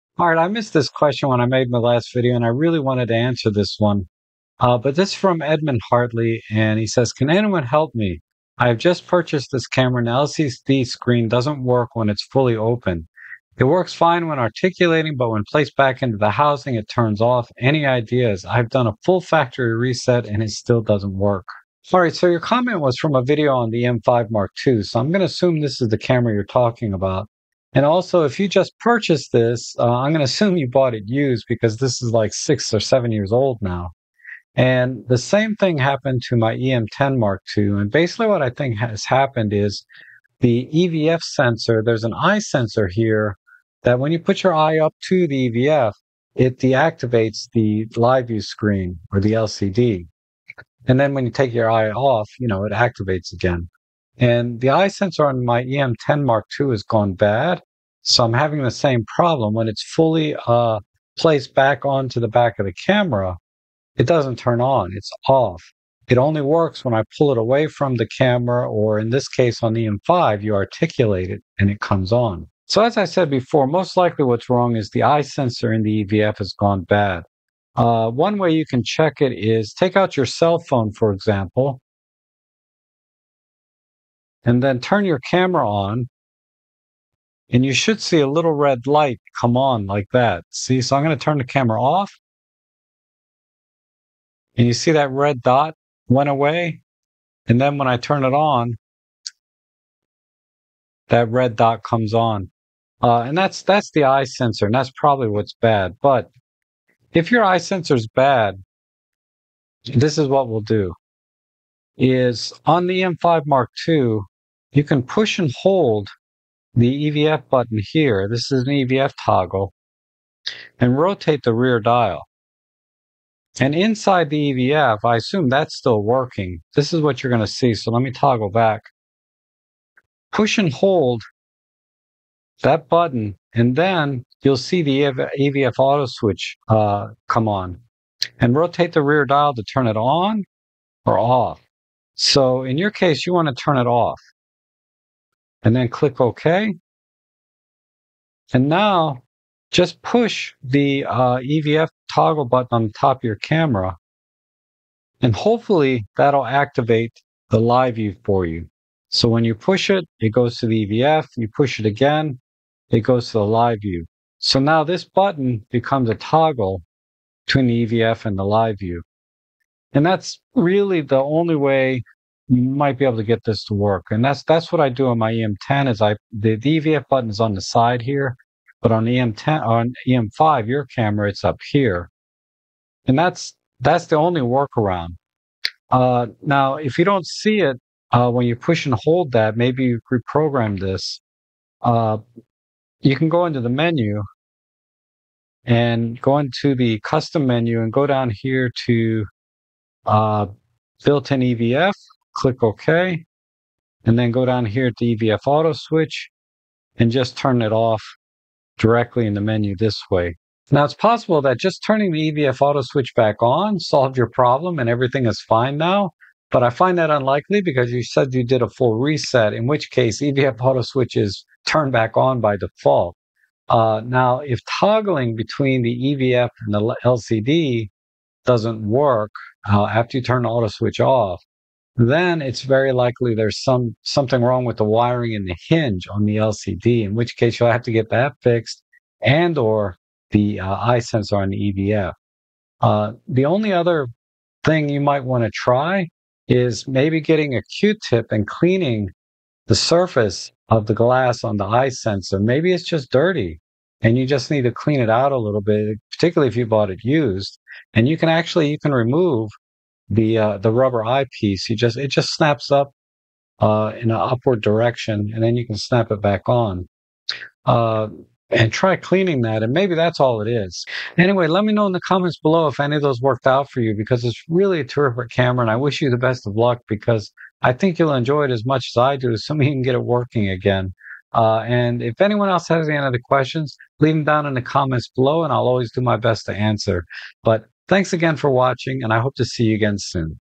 Olympus E-M5 Mark II EVF Auto Switch Not Working ep.481
00:01 @edmundhartley5489
Can anyone help me?
I have just purchased this camera and the LCD screen doesn't work went it's fully open. It works fine when articulating but when placed back in the housing it turns off.
Any ideas? I have done a full factory reset and it still doesn't work

My eBay Link ad*